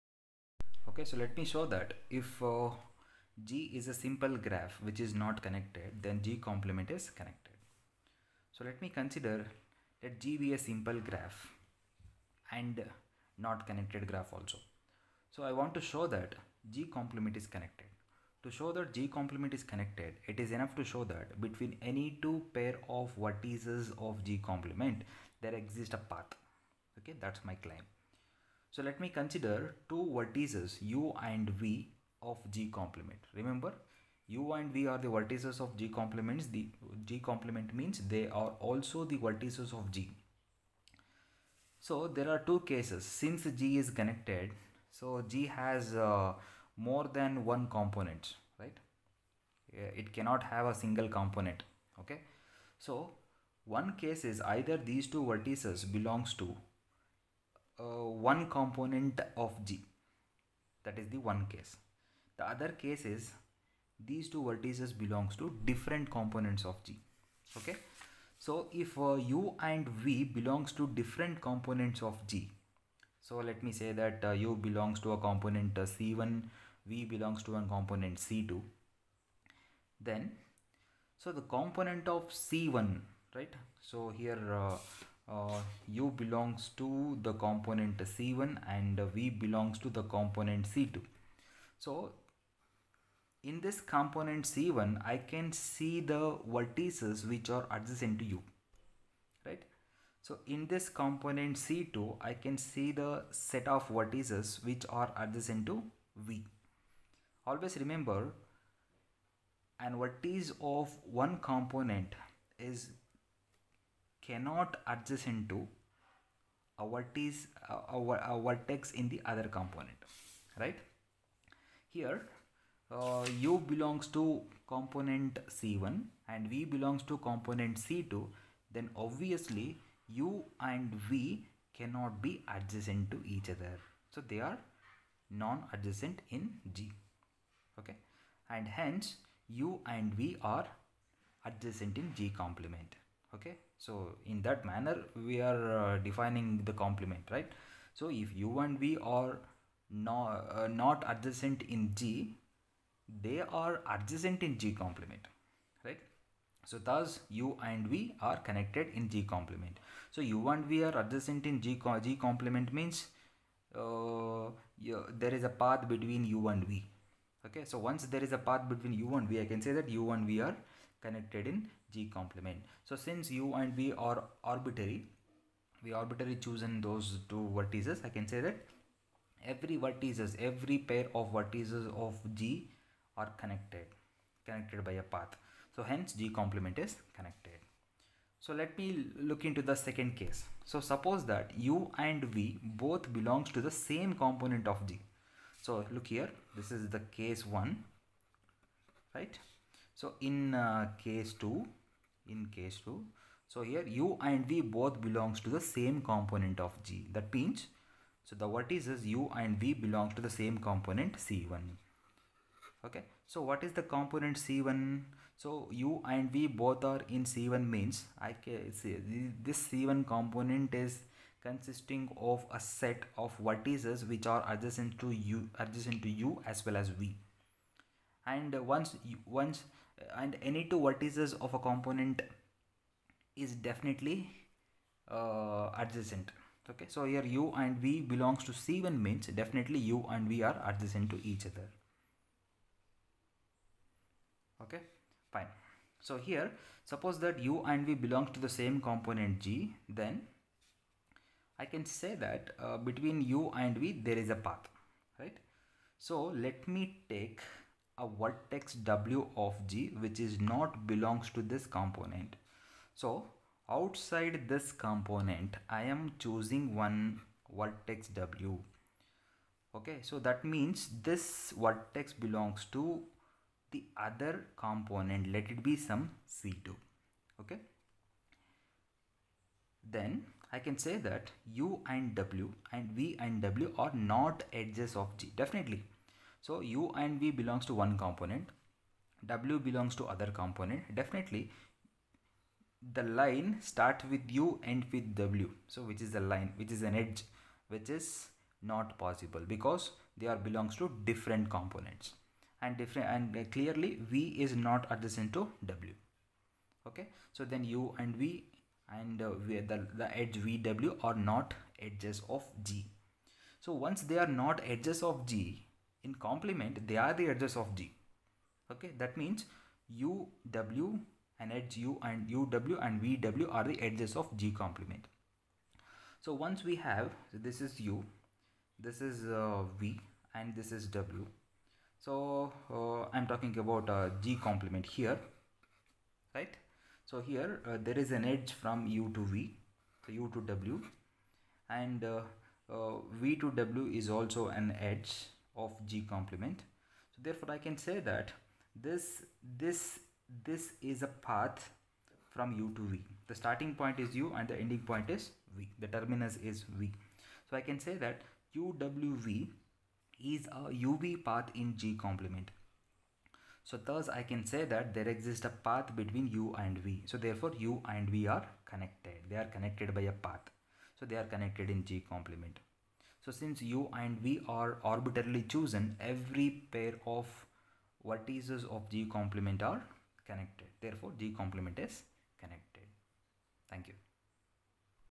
Graph Theory|Connected Graph| If a simple graph G is not connected then it's complement is connected
This playlist is very helpful for the " VTU Course Work "exam,  for M.Sc in Mathematics & also for Engineering Mathematics.
 Graph Theory - Fundamentals Explained
In this video, we explore the basic concepts of Graph Theory, an essential topic in Engineering Mathematics and Computer Science. Learn about graphs, vertices, edges, types of graphs, and real-world applications with clear explanations and examples.

🔍 Topics Covered:

Definition of a Graph

Types of Graphs (Simple, Complete, Bipartite, etc.)

Degree of a Vertex

Paths, Cycles, and Connectivity

Real-life Applications of Graph Theory